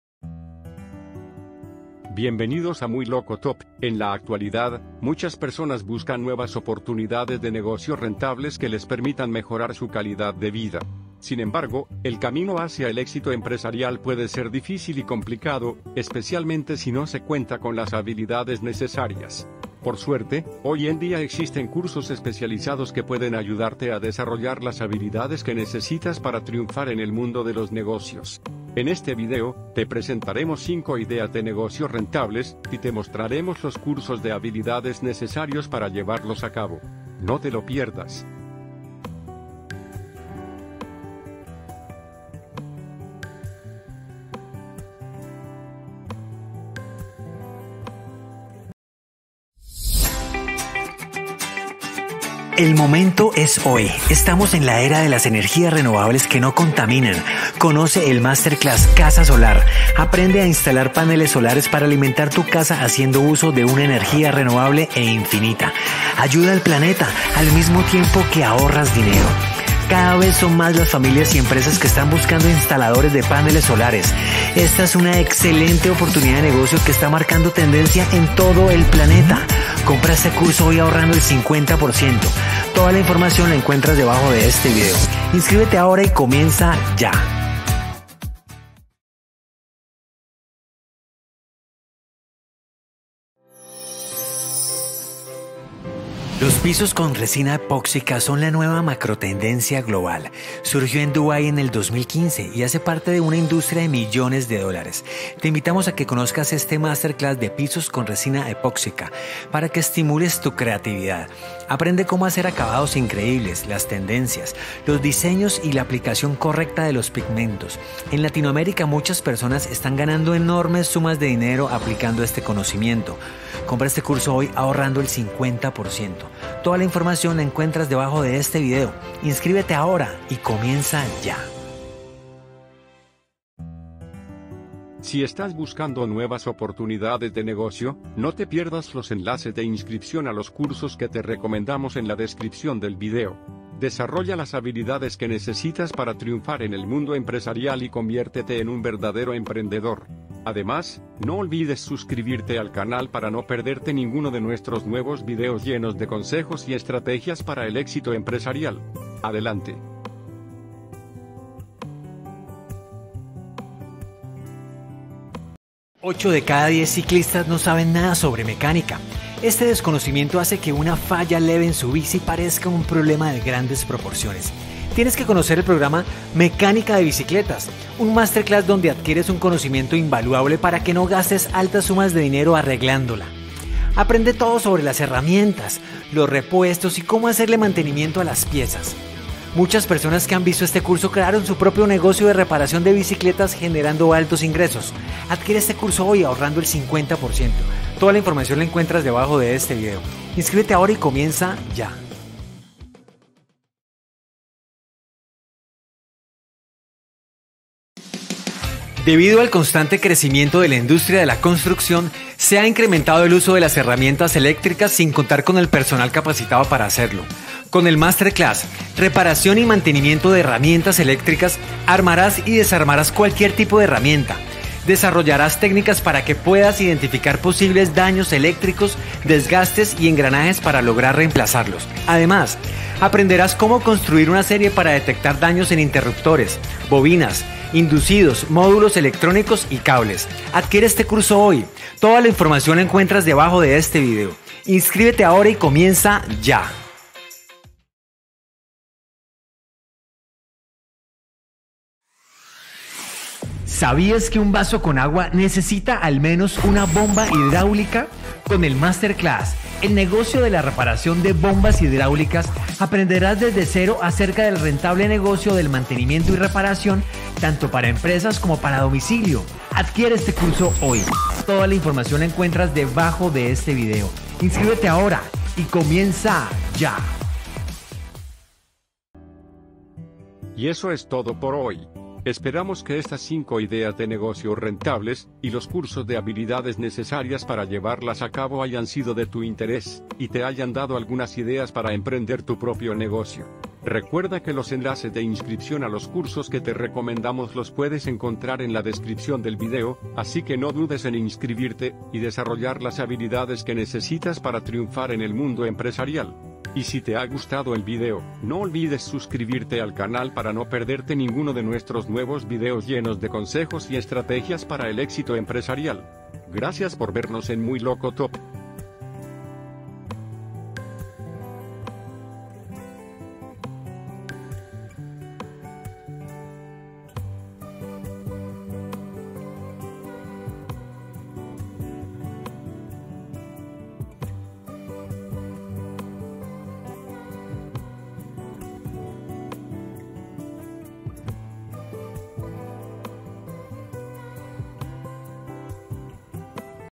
🔧🛠🔩COMO CAMBIAR TU VIDA-TOP 5 IDEAS PARA EMPRENDER EN 2023
🛠🔧😜¡Atención emprendedores! ¿Estás buscando ideas de negocio rentables y las habilidades necesarias para llevarlas a cabo? ¡No busques más! En Muy Loco Top te presentamos 5 ideas de negocio que pueden cambiar tu vida y los cursos de habilidades que necesitas para llevarlas a cabo. Desde la reparación de bombas hidráulicas hasta la instalación de paneles solares, te mostramos cómo puedes empezar tu propio negocio y triunfar en el mundo empresarial. No te pierdas este video lleno de consejos y estrategias para el éxito empresarial. ¡Inscríbete en nuestros cursos recomendados y conviértete en el próximo emprendedor exitoso! Encuentra los enlaces de inscripción en la descripción del video.

POR AQUI TE DEJO LOS ENLACES DE INSCRIPCION A LOS CURSOS
-REPARACION Y MANTENIMIENTO DE HERRAMIENTAS ELECTRICAS